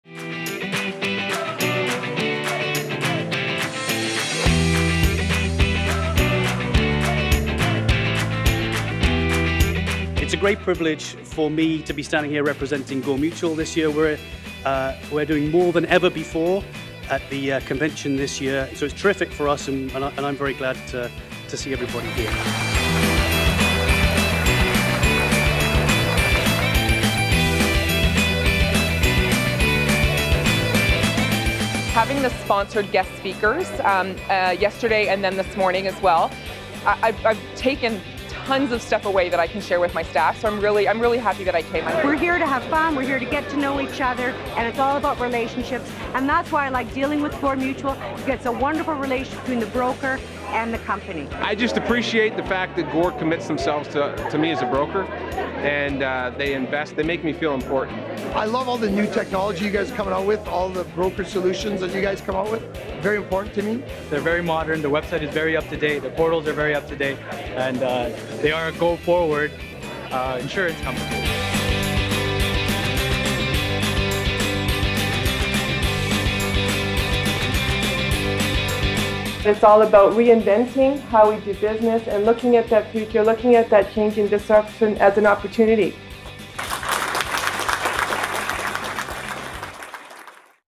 Gore Mutual at the 2016 IBAO Convention
Here are our highlights from the 2016 IBAO Convention in Toronto.

We hope you enjoyed the informative BIP talk we sponsored with BrokerLift co-founder Thomas Accardo, chatting with our staff at our new tradeshow booth, dancing and mingling at our lounge, and learning valuable business lessons from former Dragons’ Den star and disruptive entrepreneur Bruce Croxon at our Town Hall sessions.